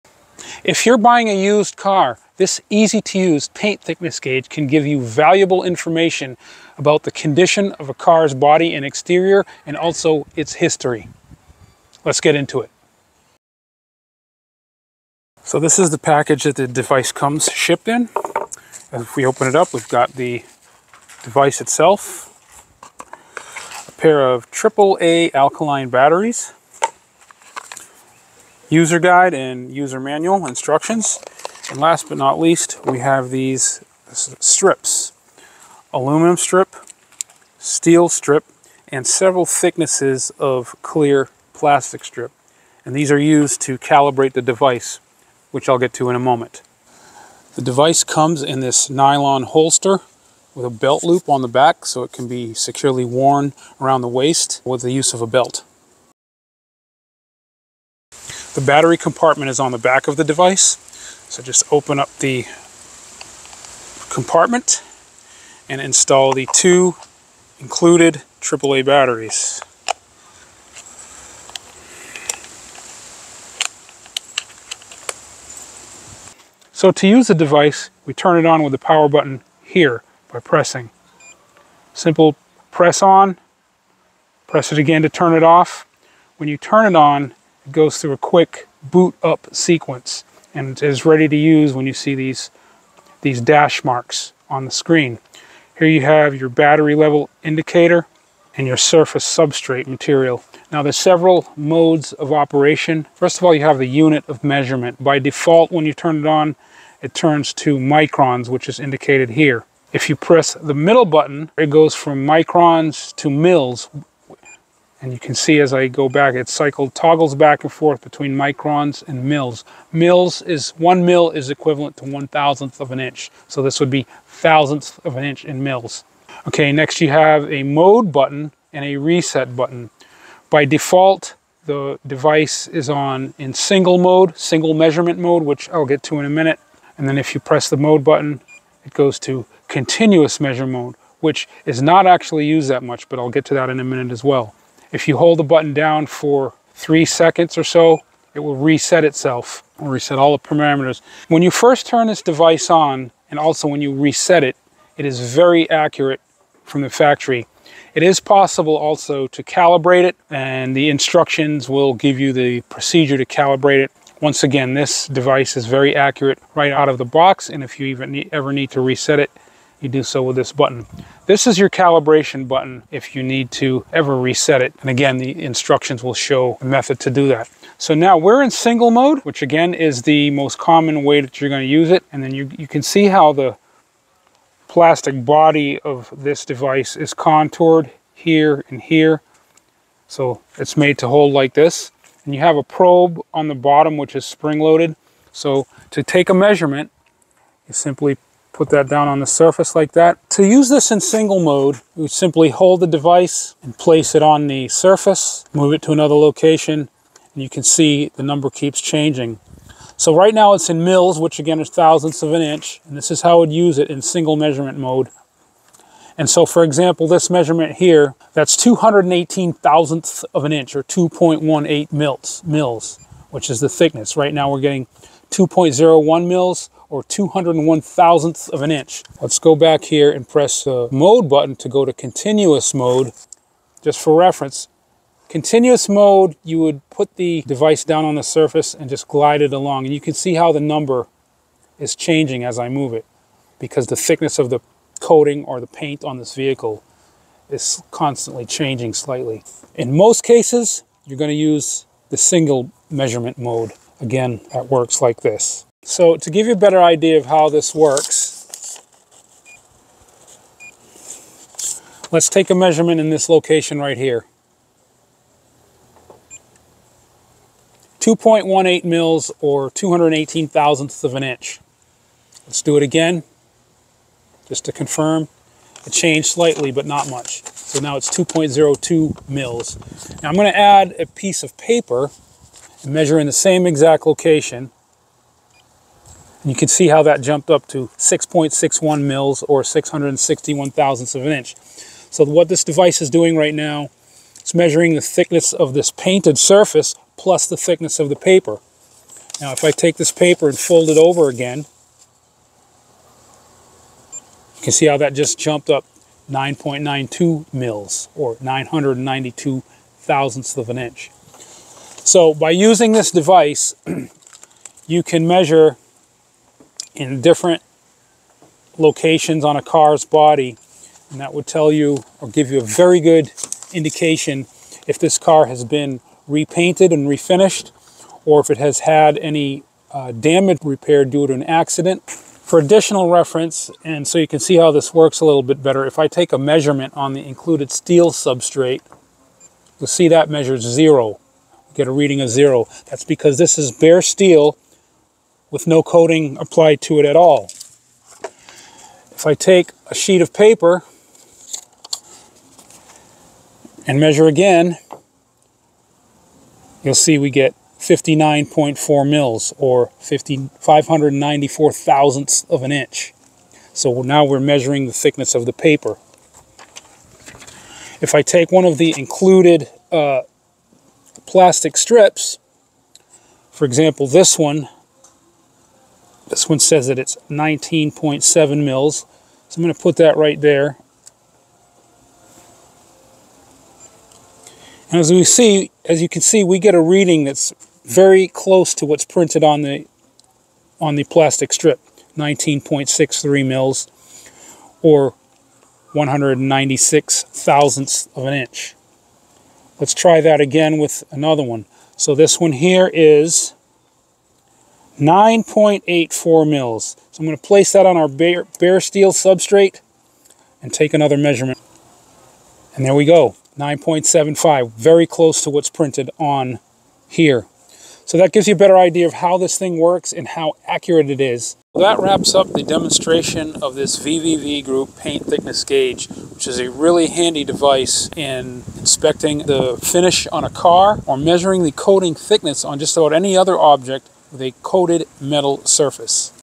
Spot hidden car body damage with this VVV CM-205FN Paint Thickness Meter
Spot hidden car body damage with this VVV CM-205FN Paint Thickness Meter. Review, setup, and demonstration.